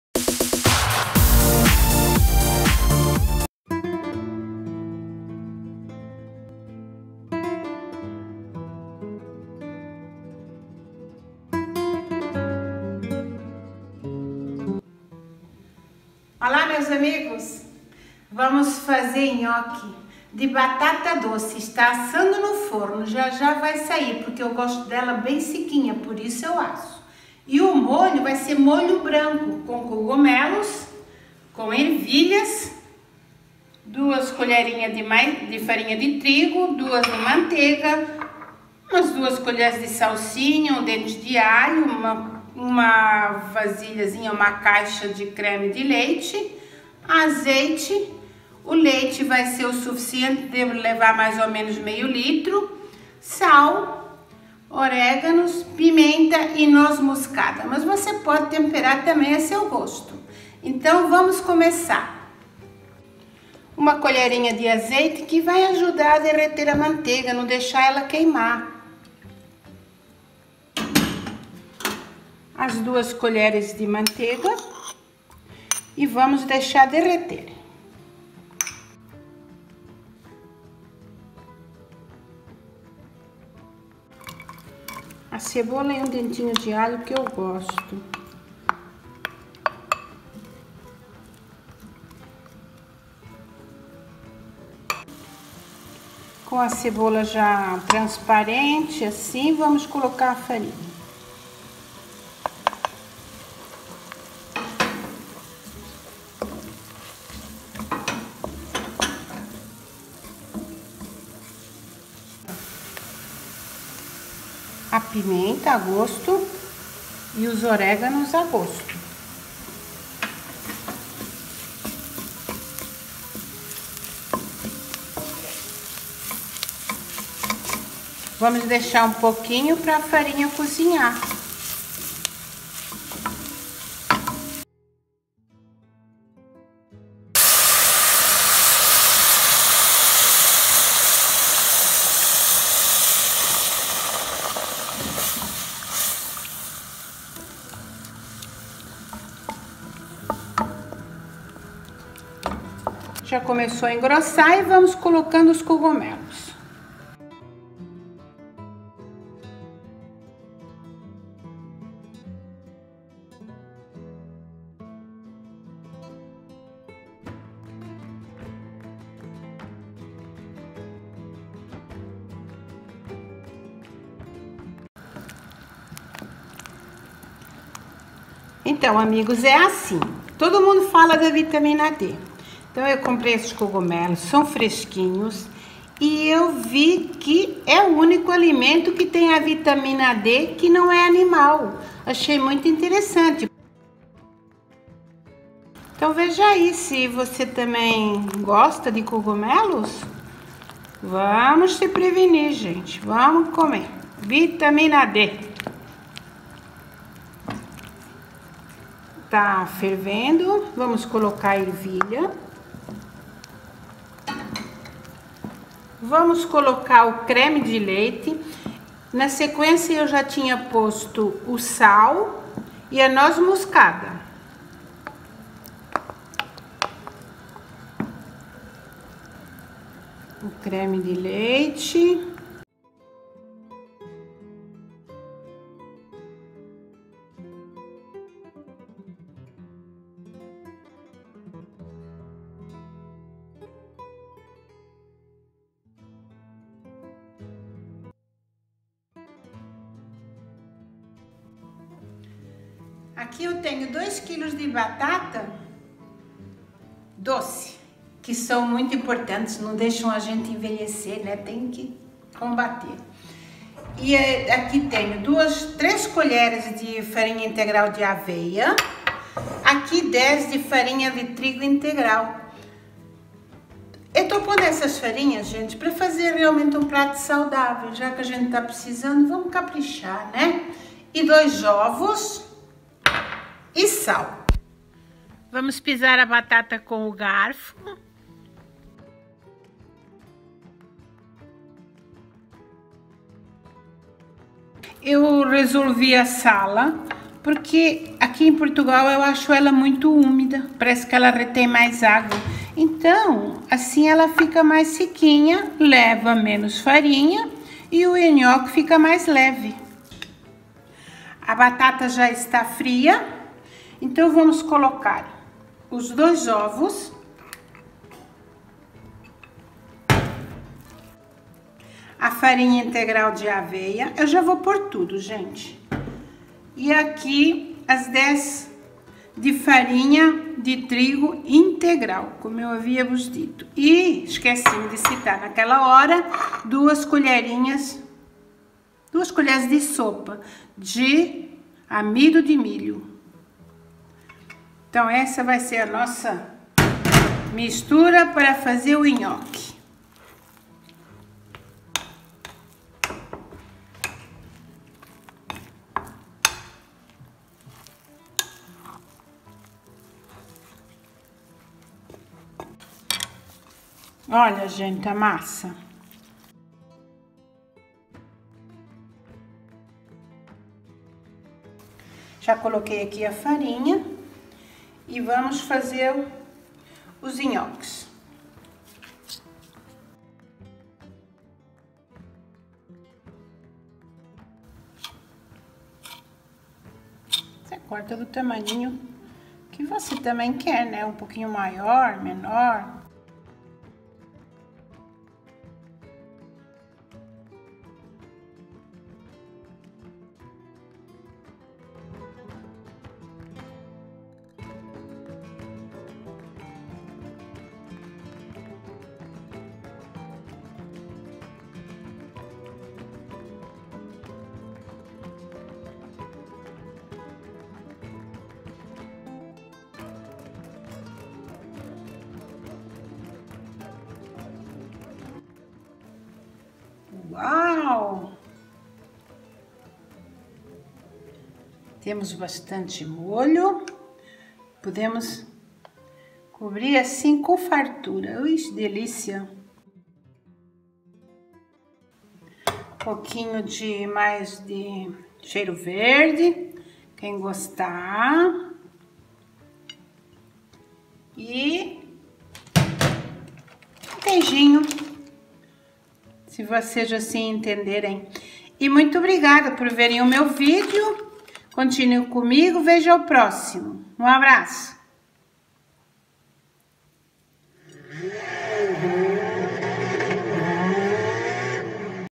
Como fazer Nhoque de batata Doce.
Como fazer  Nhoque de Batata.  Veja este prato simples e muito nutritivo com Cogumelos que contêm vitamina D.  sabores especiais  da horta, batata doce, não precisa mais apresentações é um dos alimentos mais ricos que temos e barata. Batata doce combate os radicais livres, atua contra o envelhecimento. Lá Vai Maria.
Ingredientes: " quilos de batata doce; 2 ovos ; 10 colheres de farinha de trigo integral; 3 colheres de farinha de aveia; 2 colheres de amido de milho; sal. Molho: 400 gramas de cogumelos ; meio litro de leite; Uma caixa de creme de leite; 2 colheres de sopa  de manteiga; 2 colheres de sopa de farinha de trigo 1 colher de azeite; um dente de alho e uma cebola pequena; sal, nós moscada; oréganos e pimenta a gosto. Até sabe bem na ceia de natal.